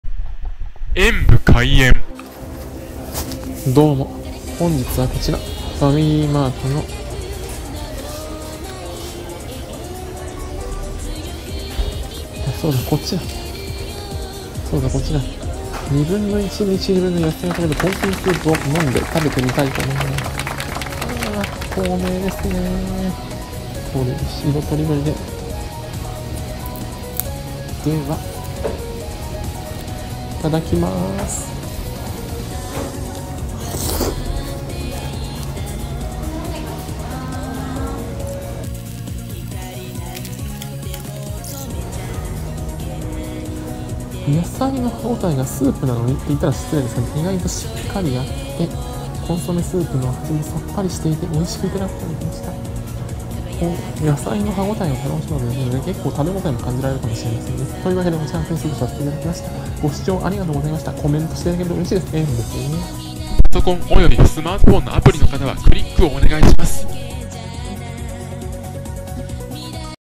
ファミリーマート 半日分の野菜が取れるコンソメスープを飲んでみた。何処か懐かしい、そんな味です……
ファミリーマート　半日分の野菜が取れるコンソメスープを飲んでみました。コンソメスープ、何処か懐かしい響きと味わい……本当にお野菜もたっぷり！！
贈り物は此方まで↓
〒153-0042